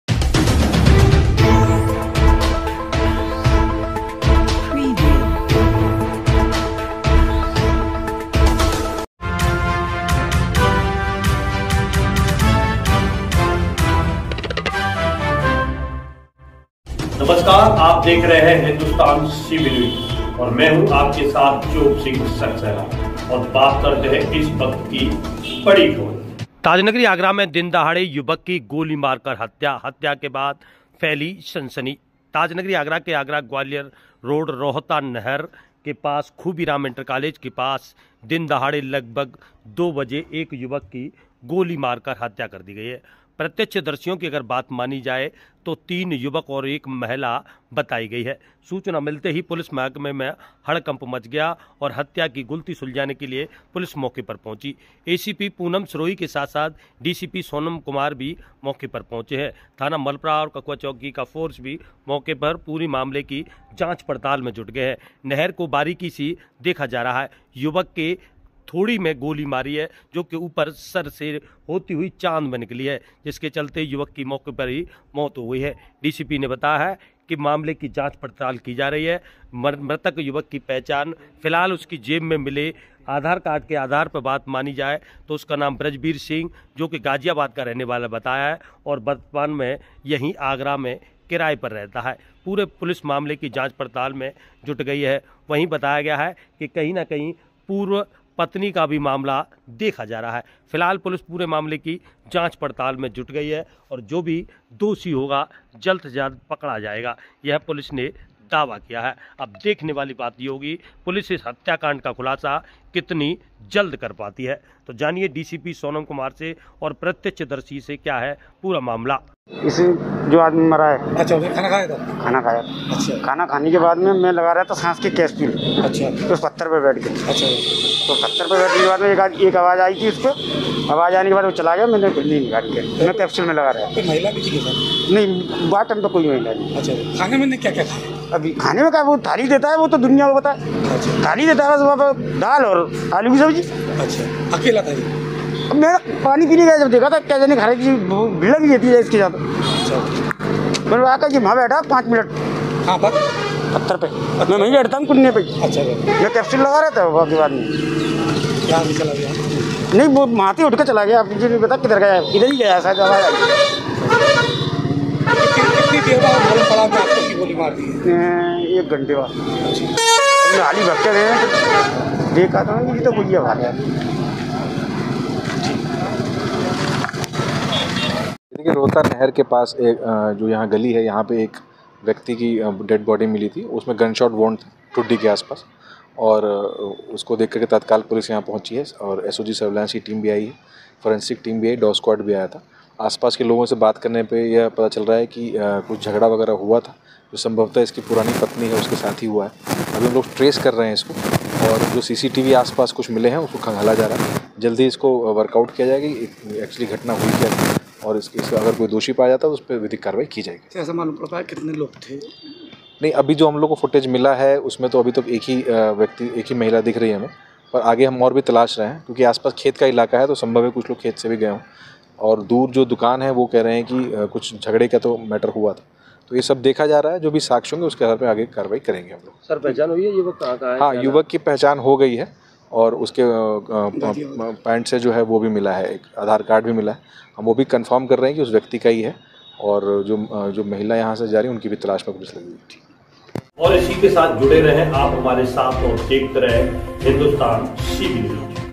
रोहता नहर चौराहे के नजदीक युवक की गोली मारकर हत्या, rohta chaurahe per Goli markar hatya
दिनदहाड़े गोली मारकर युवक की हत्या
 आशंका  व्यक्त की जा रही है कि पूर्व पत्नी का बताया जा रहा है हाथ

विवाद के चलते हुई है युवक की हत्या पुलिस कर रही है पूरे मामले की जांच पड़ताल

थाना मलपुरा क्षेत्र के रोहता नहर खूबीराम इंटर कॉलेज, के पास नहर का मामला